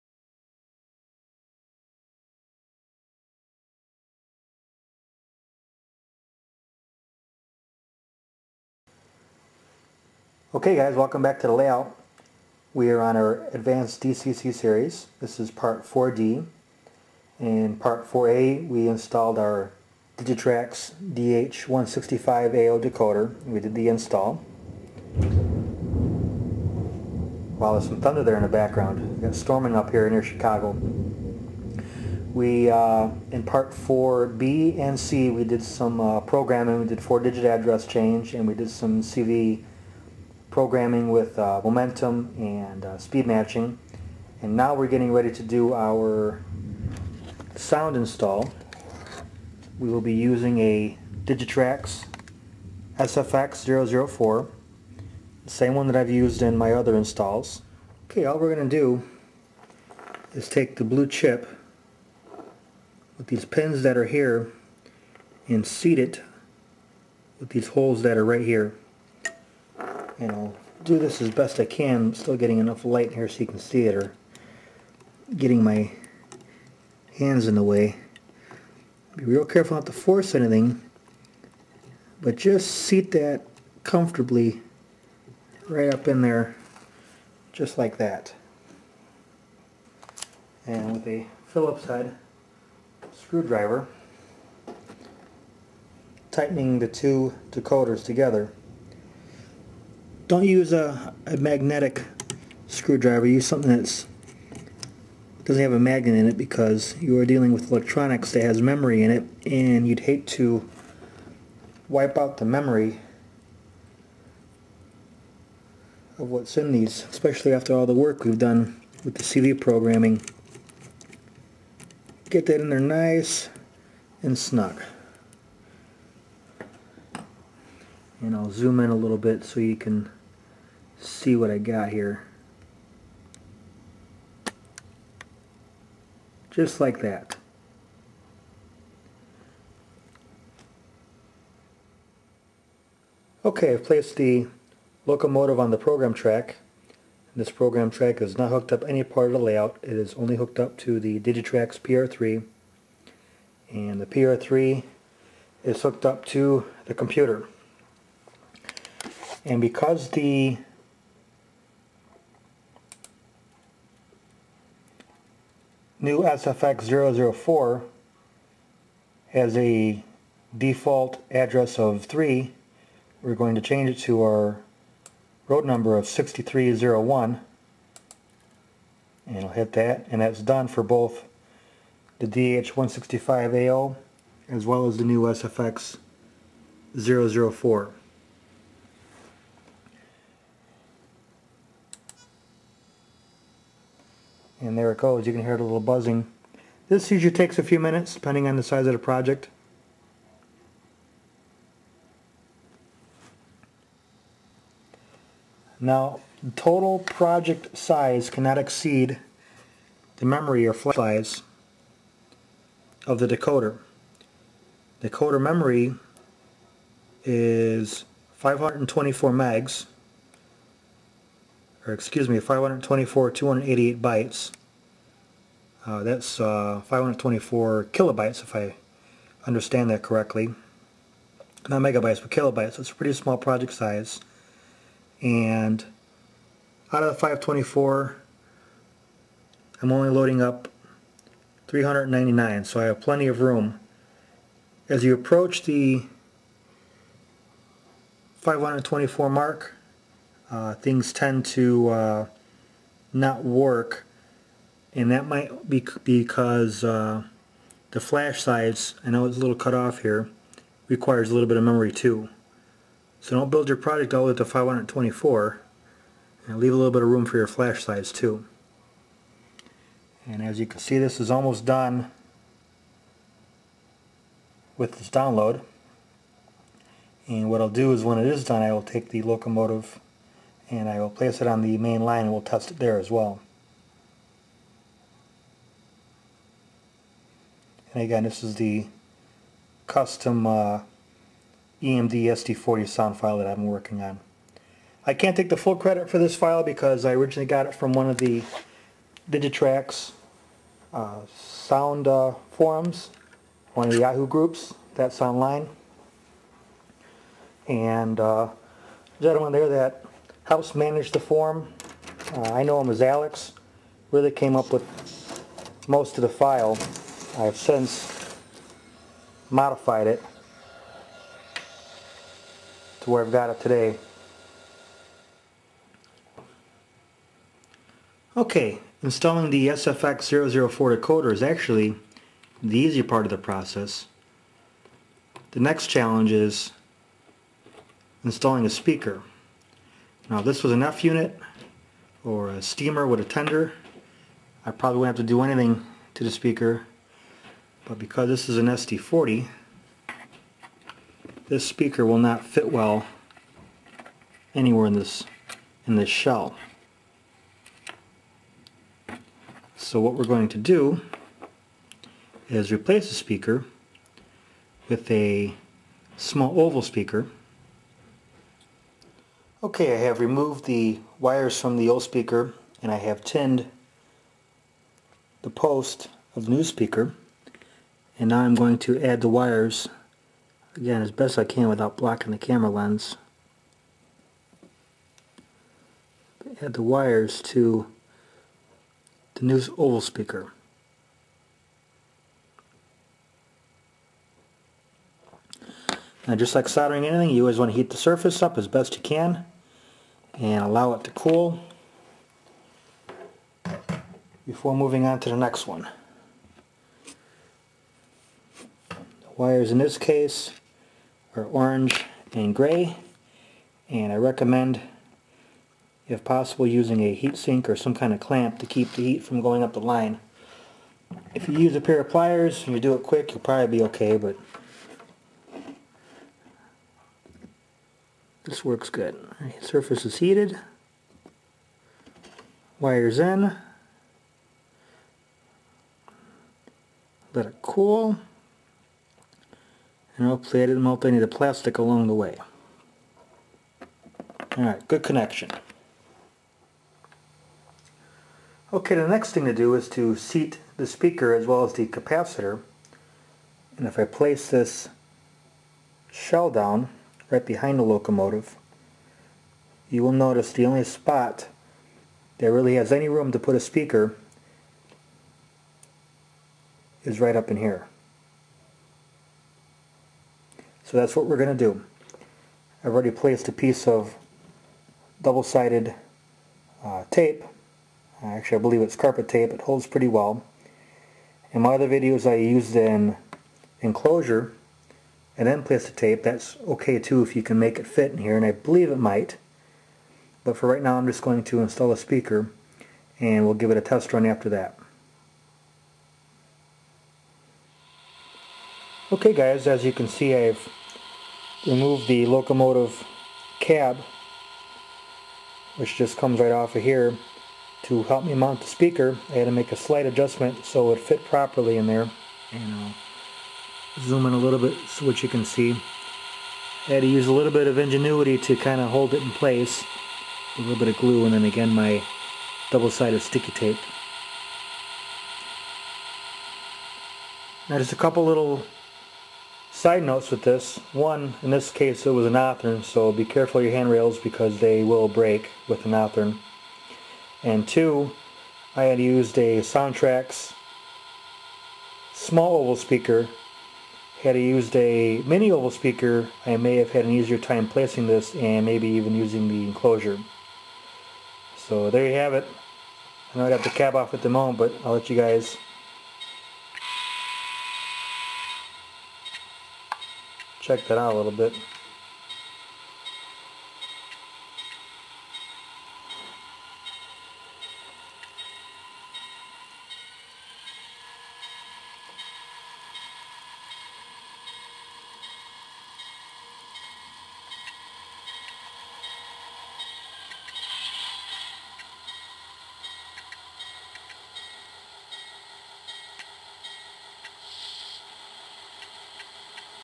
04-05-2010 Advanced DCC - Part 4D Digitrax Sound Bug Install
In this issue, we installed a Digitrax Sound Bug into an Athearn SD40 RTR. We then dowloaded some custom EMD 645 sounds